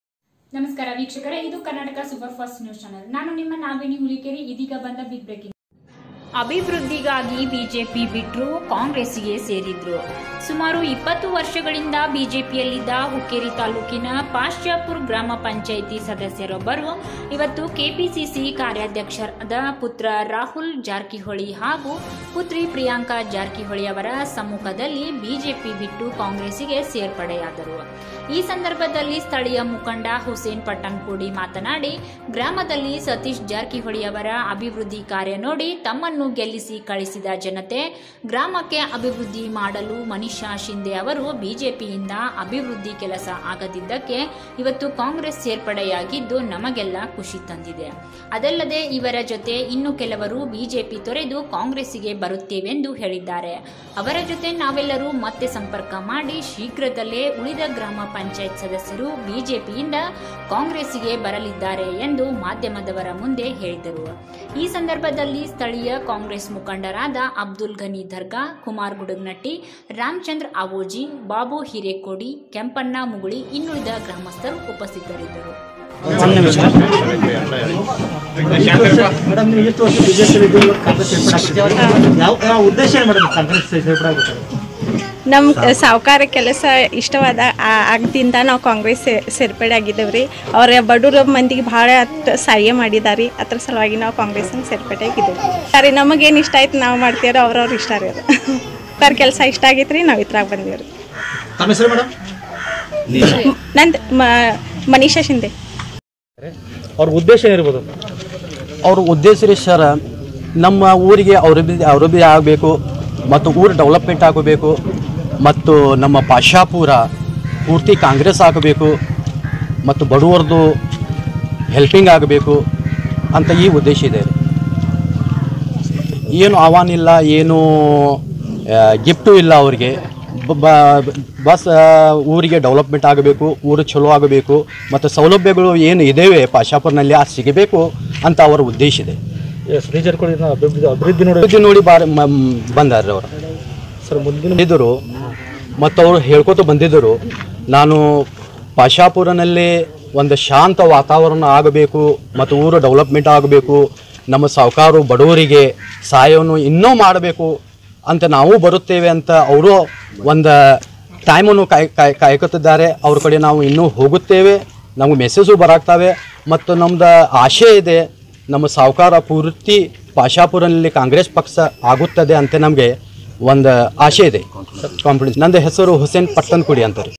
ಪಾಚಾಪುರ ಸಾರ್ವತ್ರಿಕ ಗ್ರಾಮ ಪಂಚಾಯತ್ ವಿಜಿತ ನೂತನ ಬಿಜೆಪಿ ಬೆಂಬಲಿತ ಸದಸ್ಯ ಇಗಾ ಕಾಂಗ್ರೆಸ್ ಸೇರ್ಪಡೆ.
karnatka supar fast news